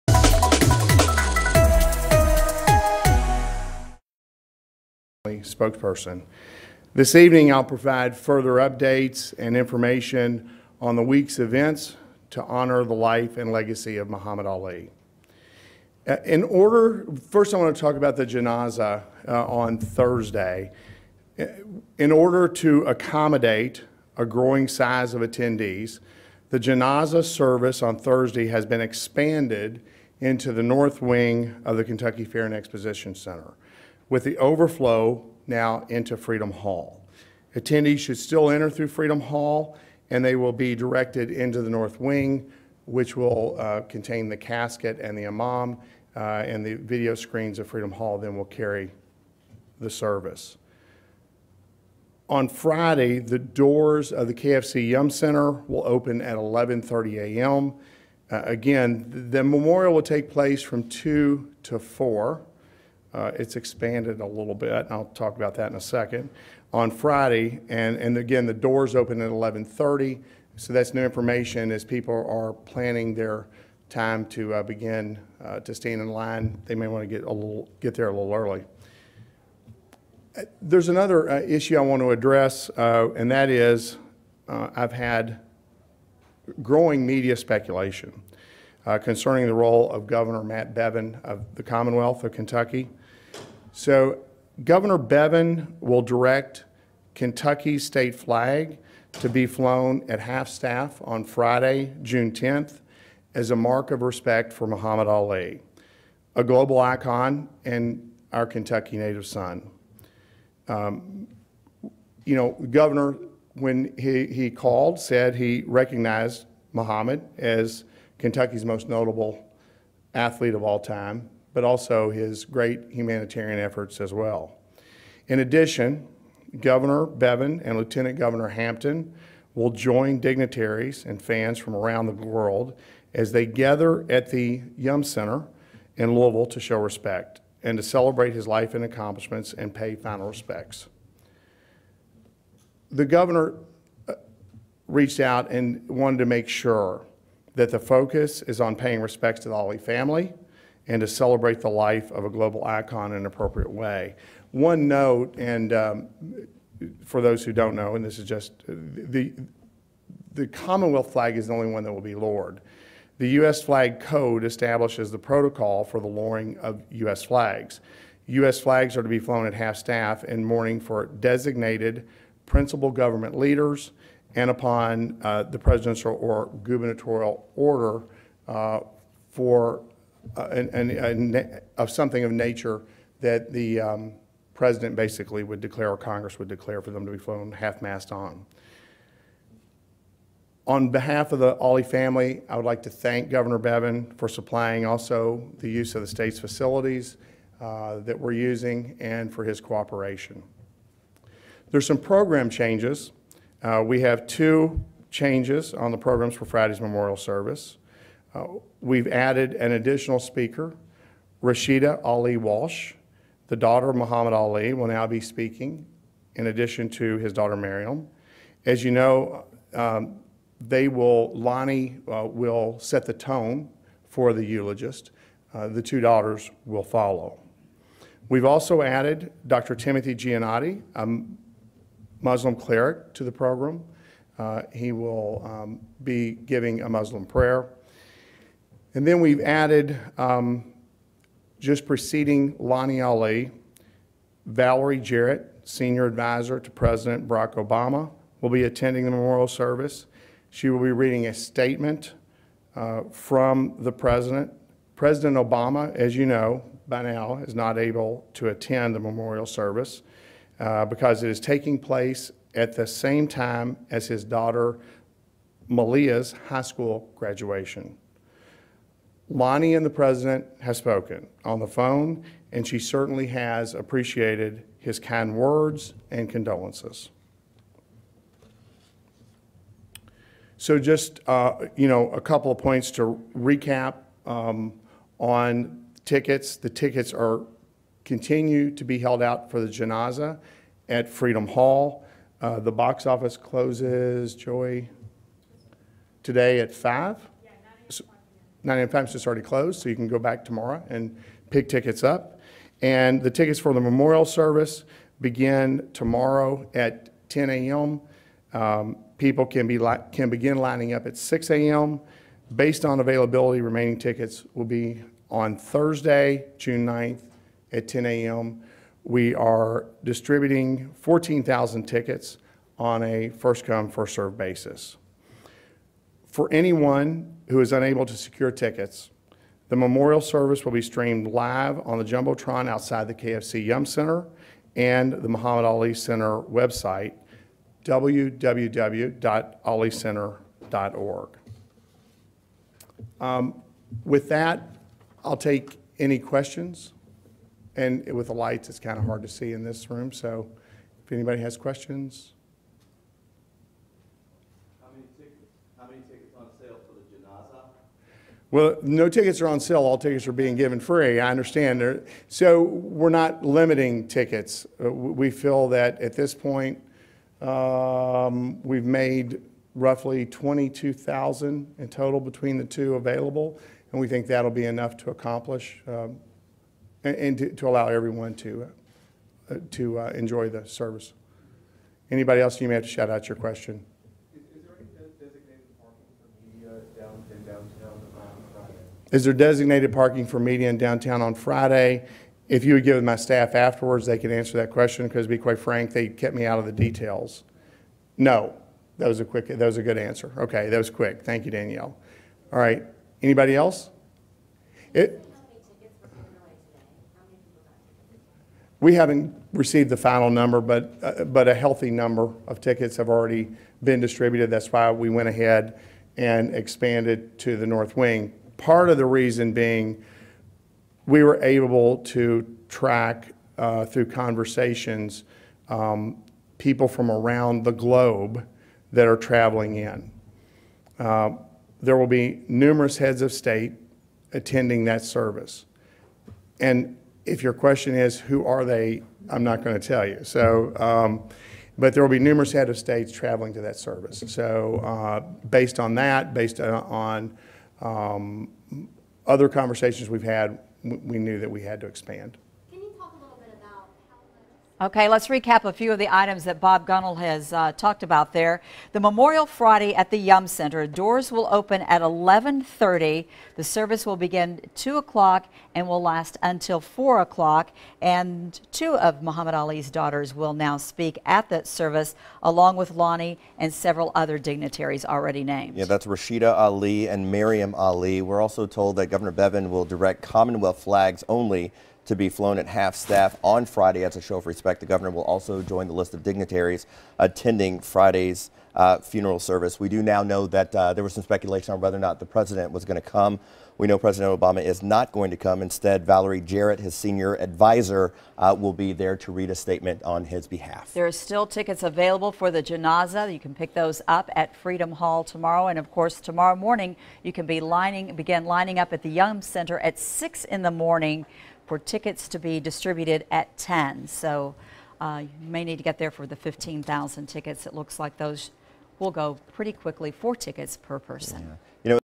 Details released on Muhammad Ali public memorial ticket information
Muhammad Ali family spokesperson Bob Gunnell releases information on Muhammad Ali public memorial ticket information.